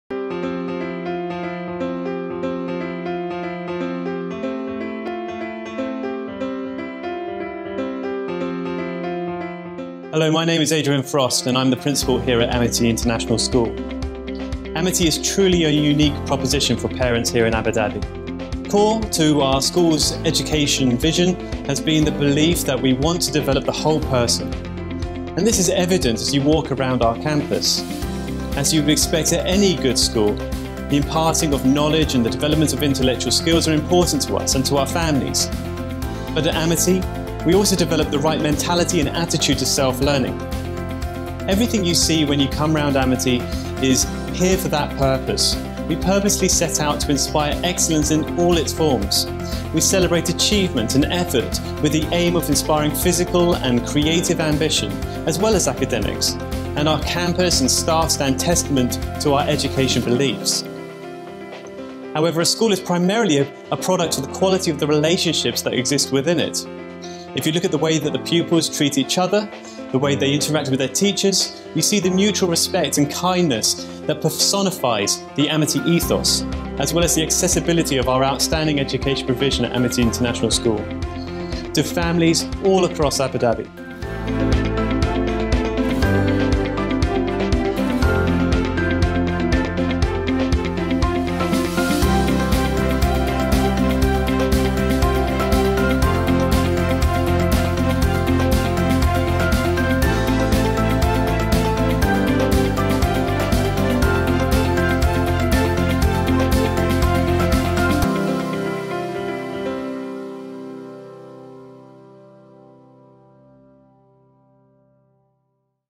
Virtual Campus Tour With Principal Adrian Frost
ExtraordinaryAmity is uniquely designed to inspire excellence in all its forms!

Take a look around our world-class facilities to see why Amity is the right choice for your family.